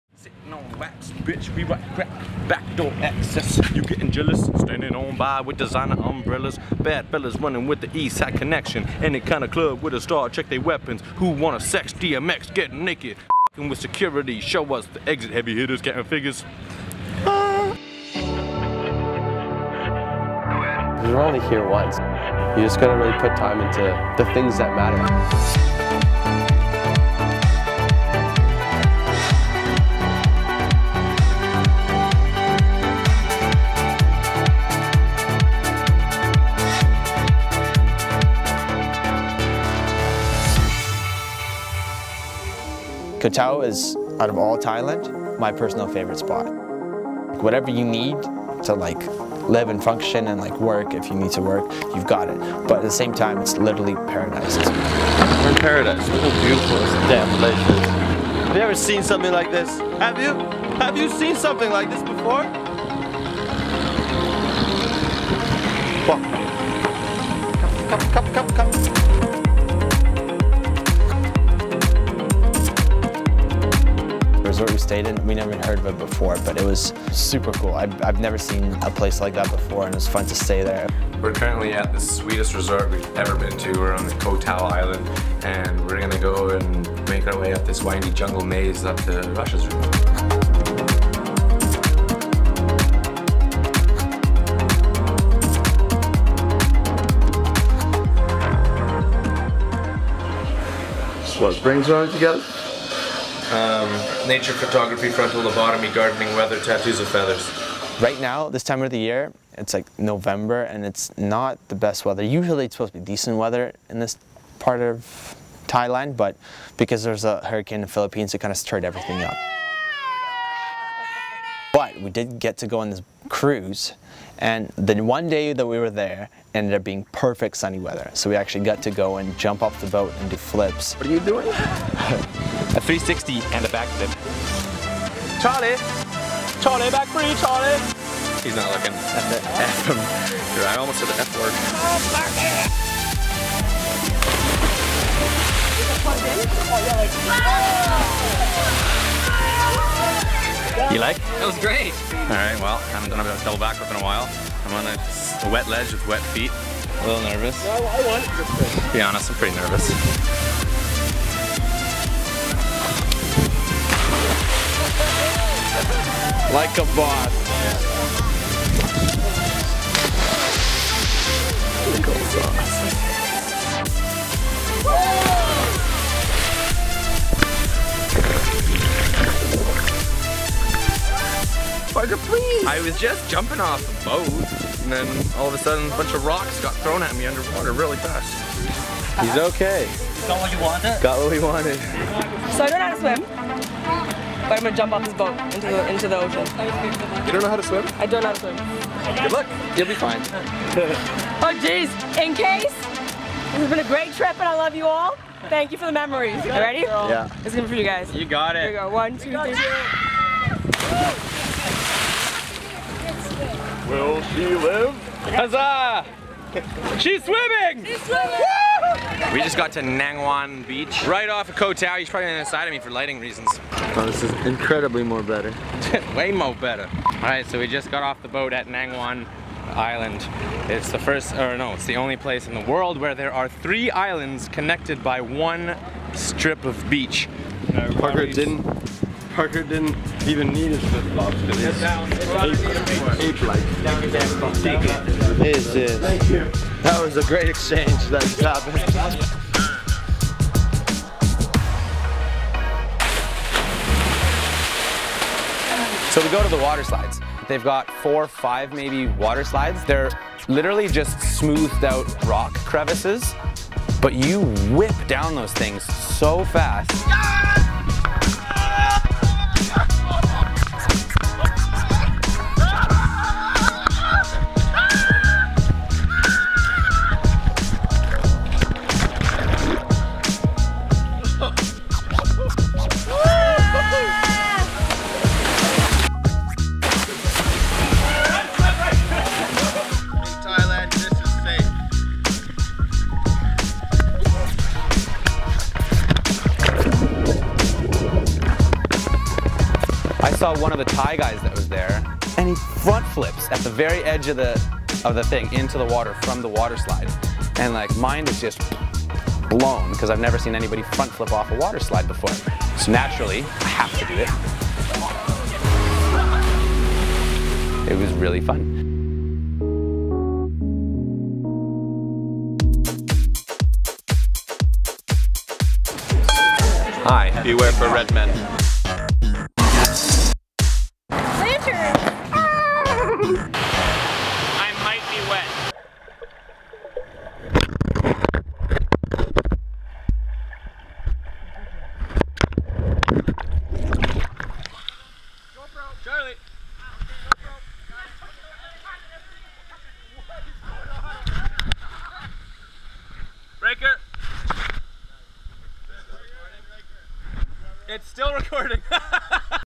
Thailand Water Sports (Ch 22)
to see more of our travel adventures. We visited Thailand and Koh Tao for the third time, this time on a Contiki Press trip with a bunch of awesome sauce YouTubers!